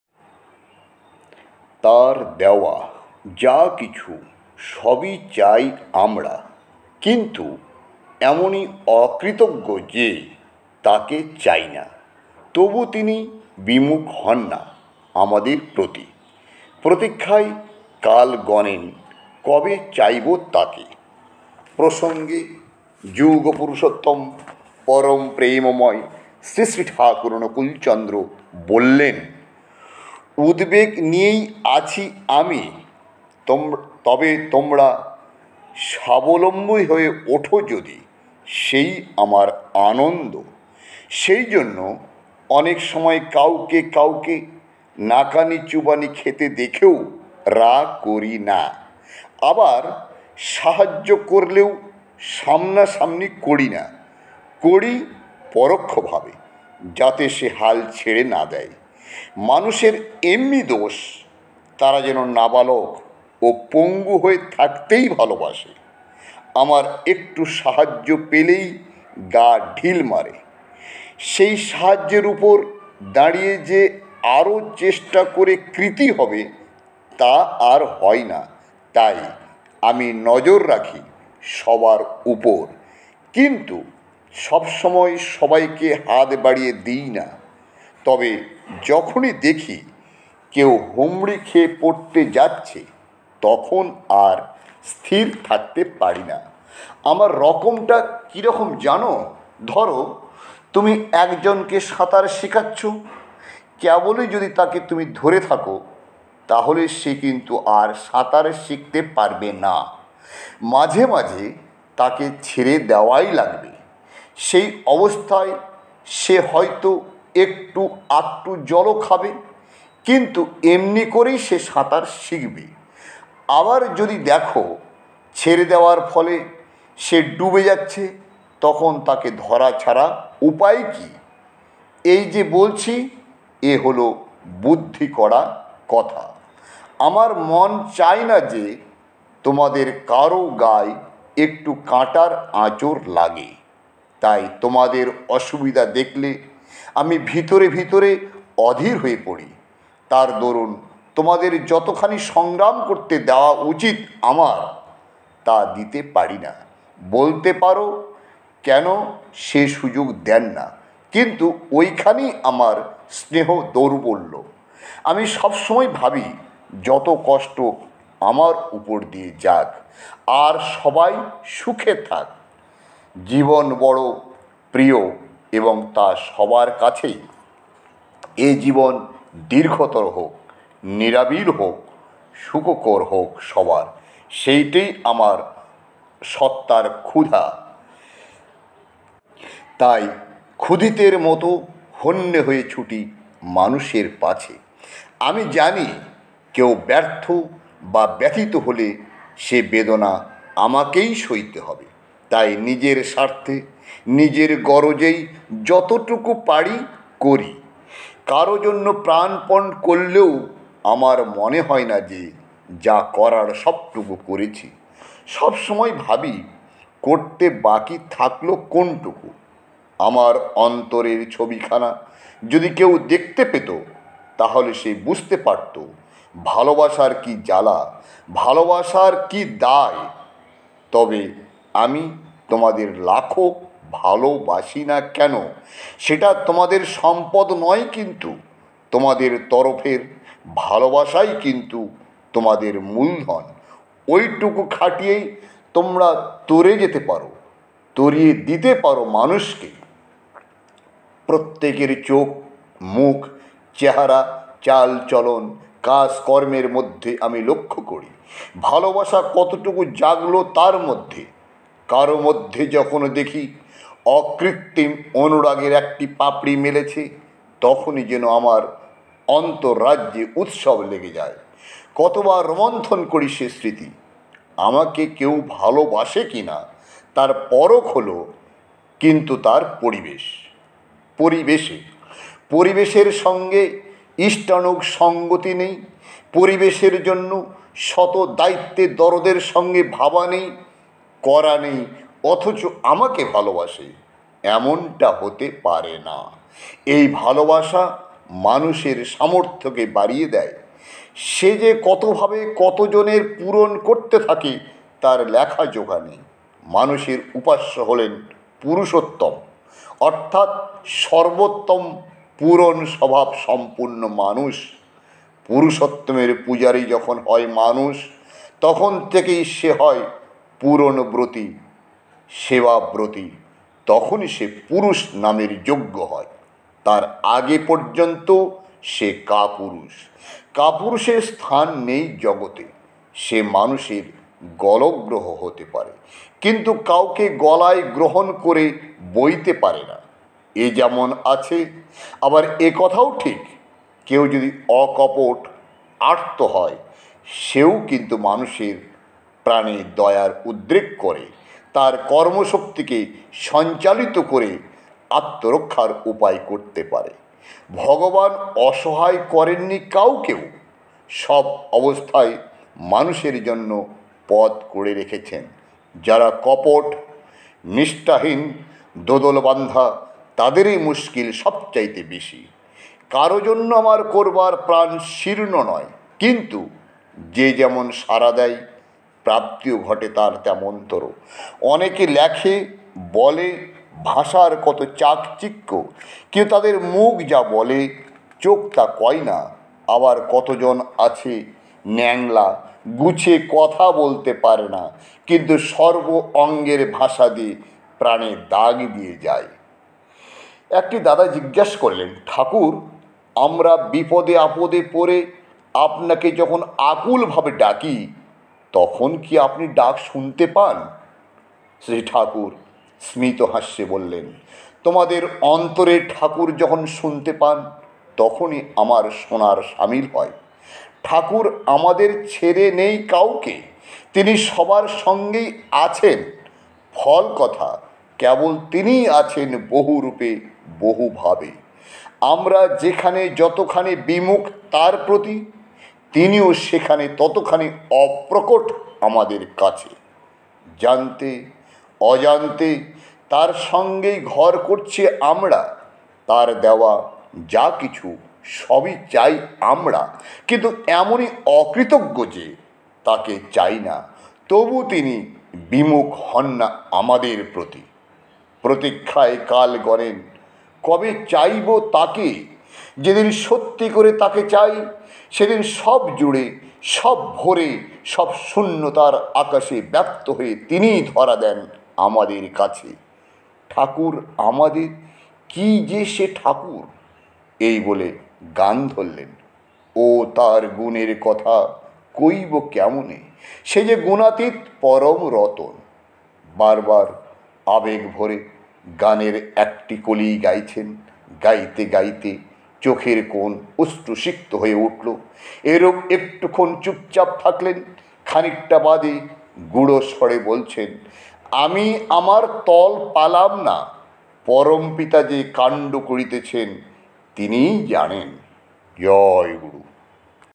প্রসঙ্গে পরমপ্রেমময় শ্রীশ্রীঠাকুর অনুকূলচন্দ্র
তাঁর দেওয়া যা'-কিছু সবই চাই আমরা। কিন্তু এমনই অকৃতজ্ঞ যে,তাকে চাই না, তবু তিনি বিমুখ হন না আমাদের প্রতি। প্রতীক্ষায় কাল গণেন, কবে চাইব তাঁকে।
সূত্র-আলোচনা, আষাঢ়, ১৪২৯, পৃষ্ঠা-১৪০-১৪২